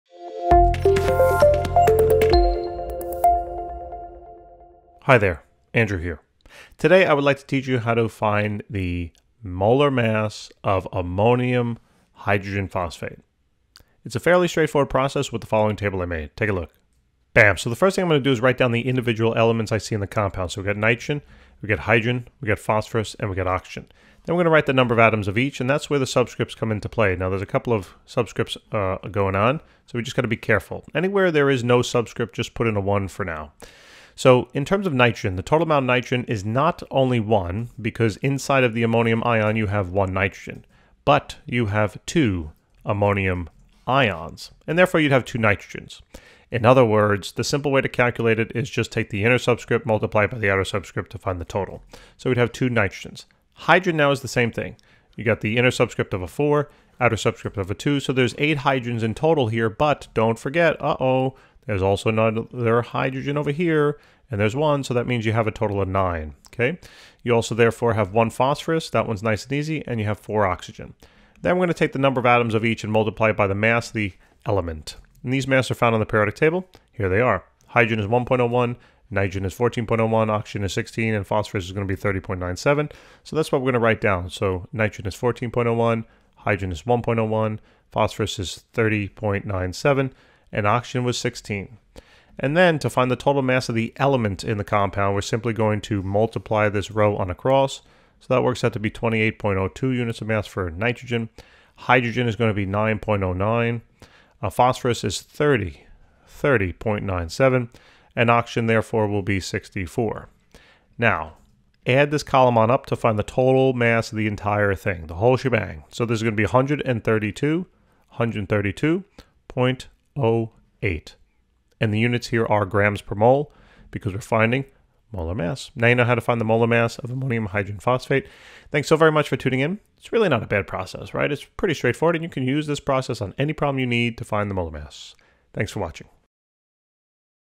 How to find the molar mass of (NH4)2HPO4 (Ammonium Hydrogen Phosphate)
Calculate the molar mass of the following:
(NH4)2HPO4 (Ammonium Hydrogen Phosphate)